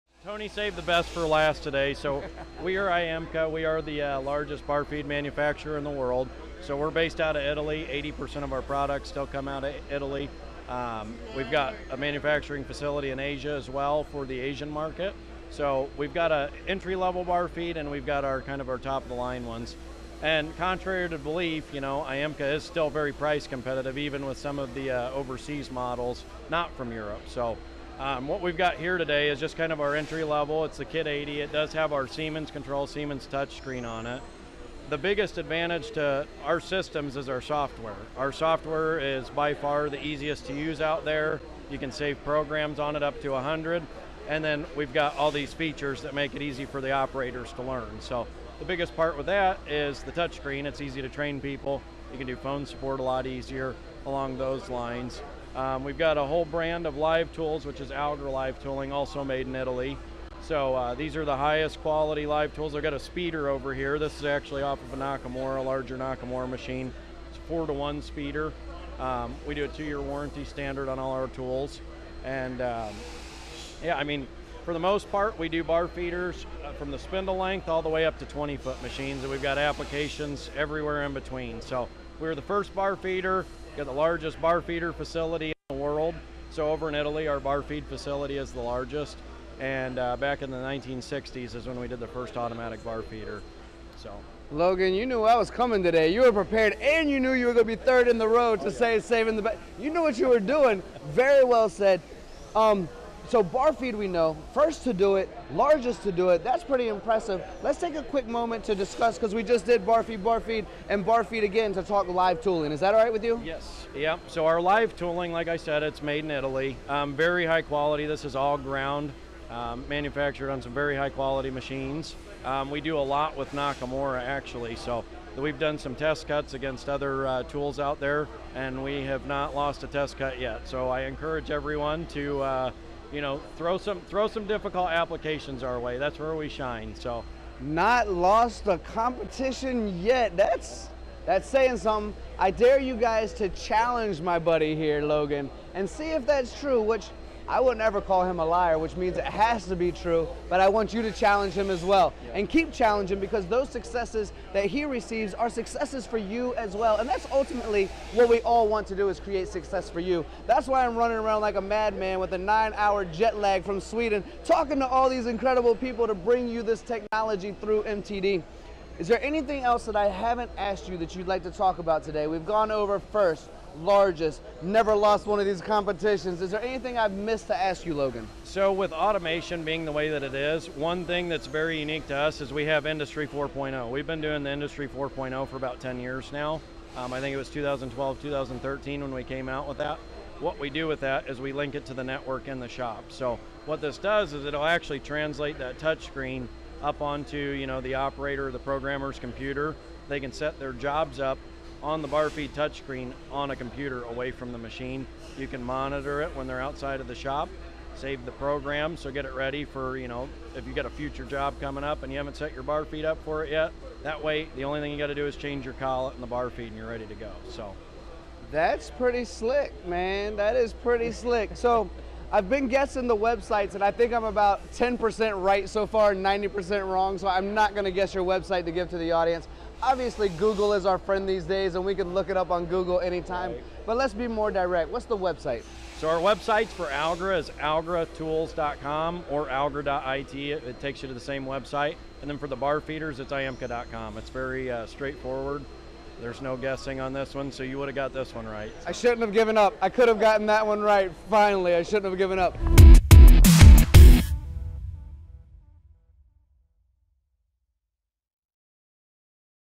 All-round manufacturing capabilities with IEMCA at Methods Machine Tools Open House
As the largest bar feed manufacturer in the world with 80% of their products coming from Italy, how is IEMCA still so price competitive in the market? Join Tony of MTDCNC and Logan of IEMCA to understand the unique and all-round manufacturing capabilities of IEMCA technology. From entry-level bar feeds and easy software to top-of-the-line products and live tooling, IEMCA’s technology is impressive. For a company that’s never lost a test cut with other tools, IEMCA could be your solution for difficult applications! Find out more from Logan about IEMCA’s role in automation and industry 4.0 and how the world leader in bar feed is bringing new technology to the table! for more details.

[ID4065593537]